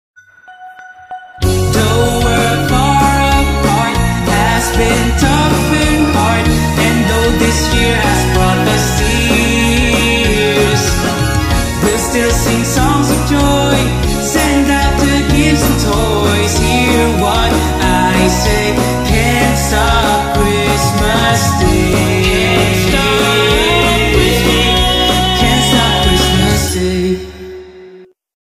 WBChristmasJiingle by Louie Miguel Manalansan (3RD PLACE WINNER)
Out of the many who sent in their entries in our WBChristmasJingle contest, we had to choose 10 finalists and had a hard time picking the winners! Congratulations to Vernix Magayanon and the other 9 who have put in their time and effort in making this. You really CantStopChristmas! Watch their amazing entries on the playlist and enjoy.